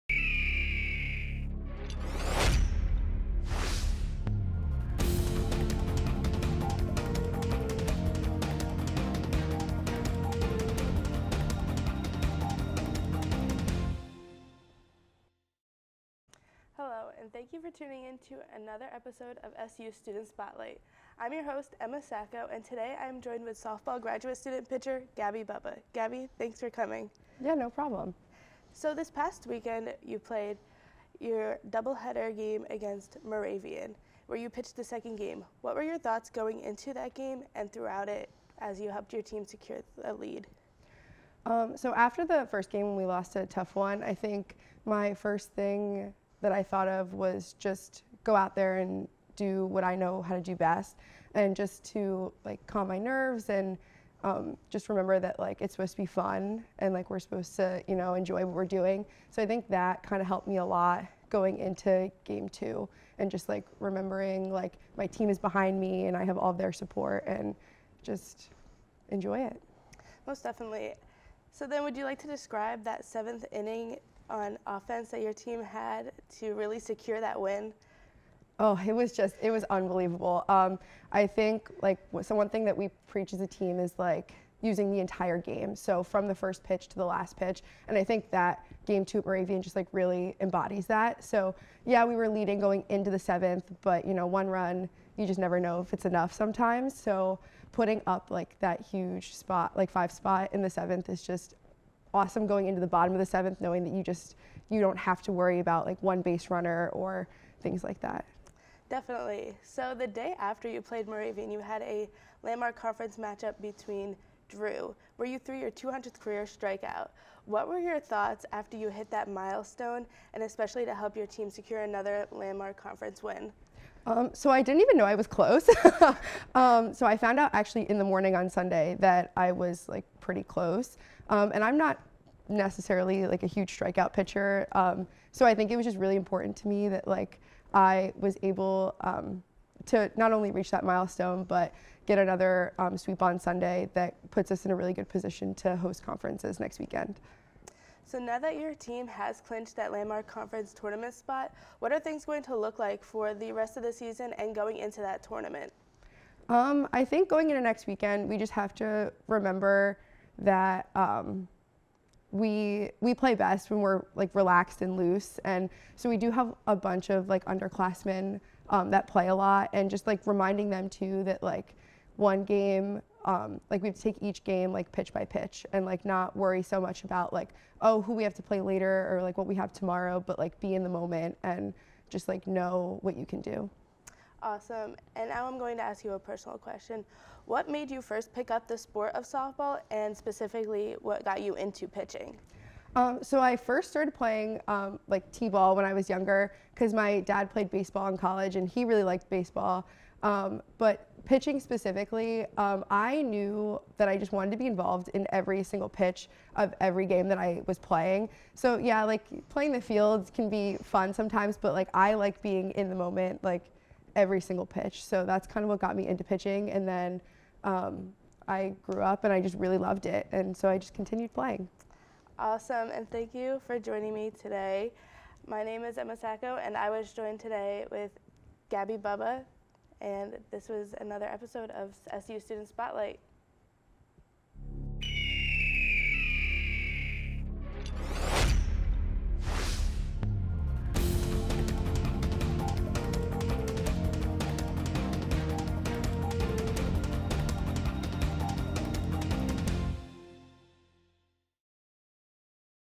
Sport Spotlight - Gabby Bubba Women's Softball
Emma Sacco interviews Gabby Bubba from the women's softball team about their current season.